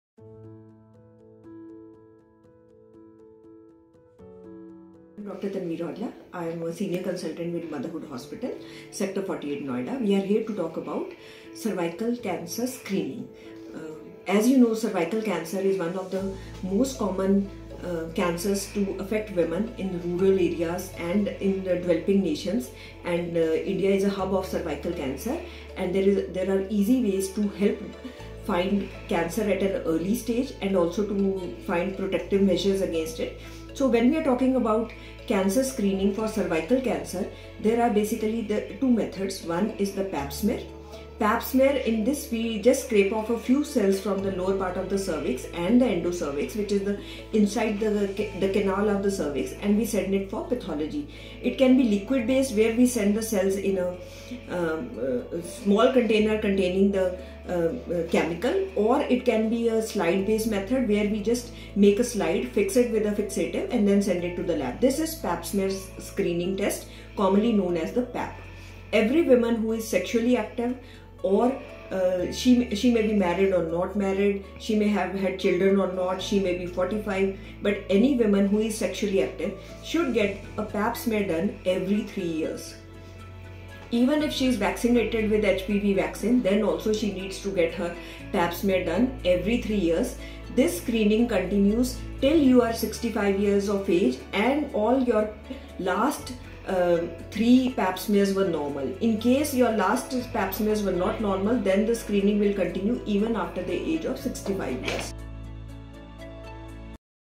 Pap Smears: Your First Line of Defense Against Cervical Cancer - Insights from Dr. Tanveer Aujla
Prevention is better than cure, especially when it comes to cervical cancer.

Listen to Dr. Tanveer Aujla as she breaks down the importance of pap smear tests in detecting cervical cancer at early stages.

She also sheds a light on who should prioritize these tests and how often should they go for it.

Get in touch with us about any query on 𝟭𝟴𝟬𝟬 𝟭𝟬𝟴 𝟴𝟬𝟬𝟴

Motherhood Hospitals have some of the best women and child care specialists around the country, to know more about their experience